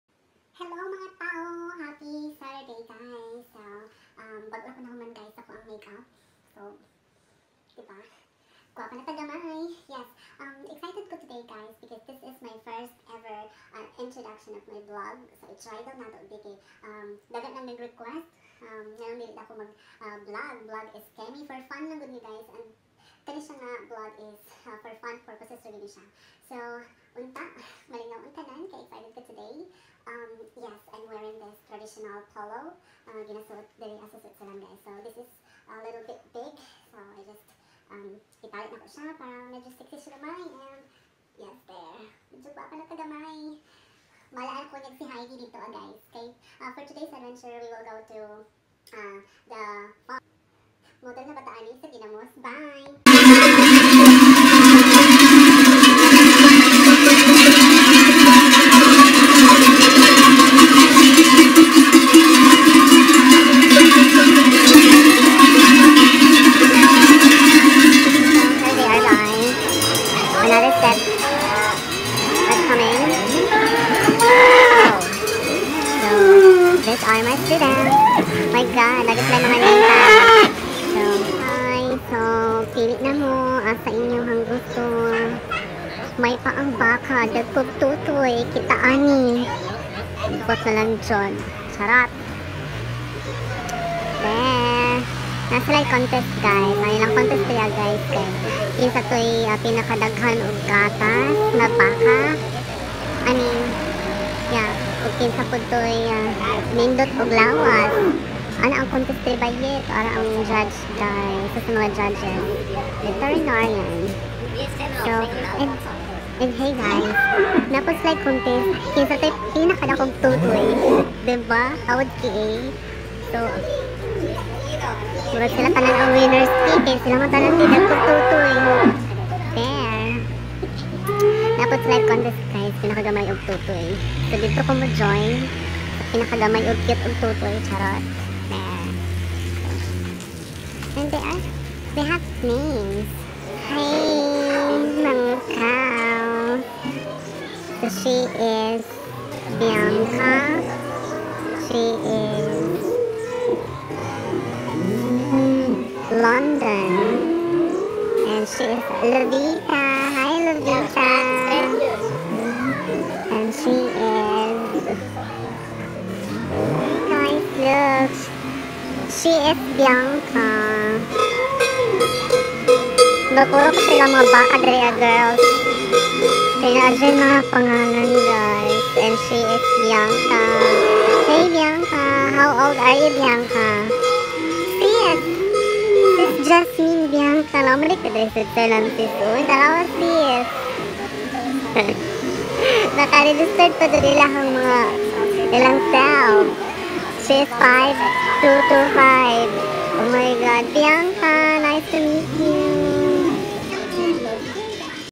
Life in Switzerland - My first ever Vlog ♥️ 💕🇨🇭
Its Farmers day here inSeitzerland 💕🇨🇭